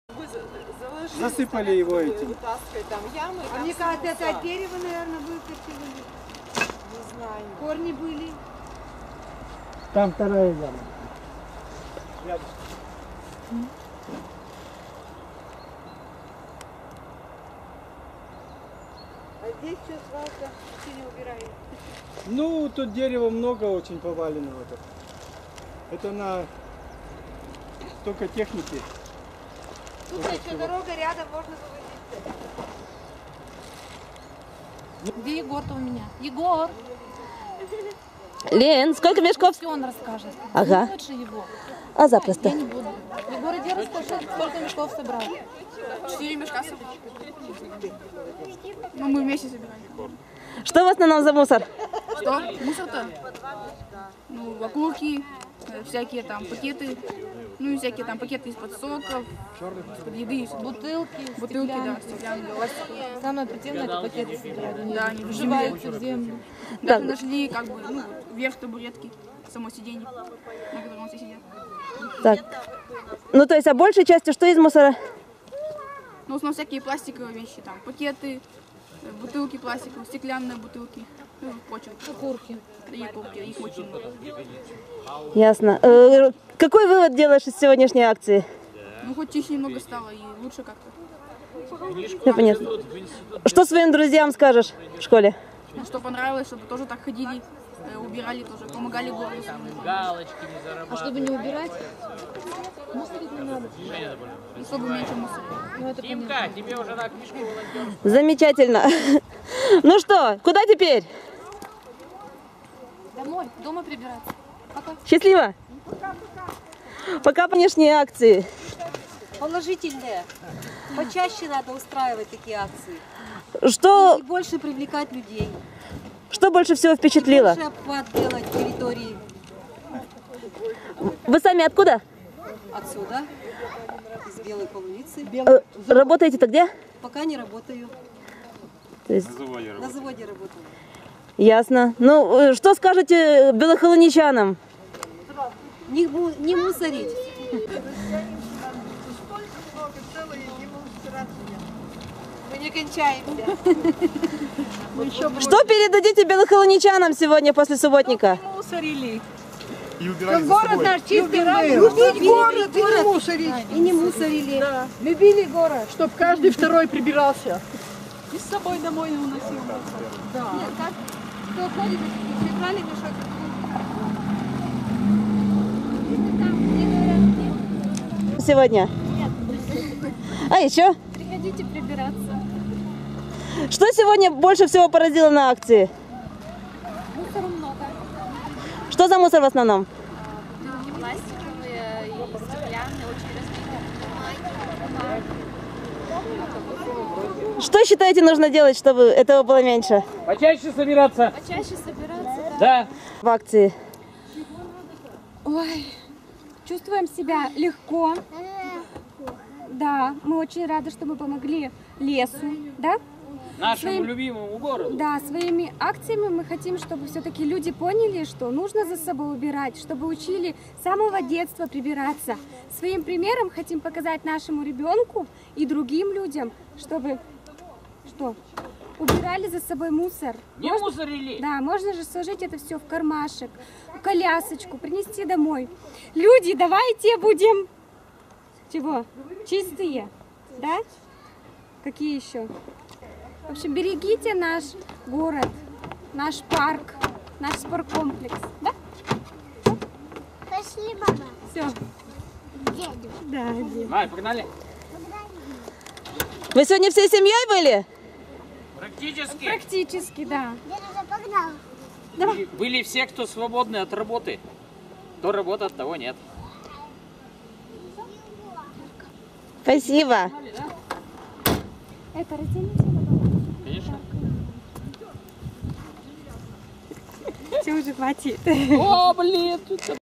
Впечатления участников субботника "Зеленая Россия"
Участники акции "Зеленая Россия" в Белой Холунице делятся впечатлениями.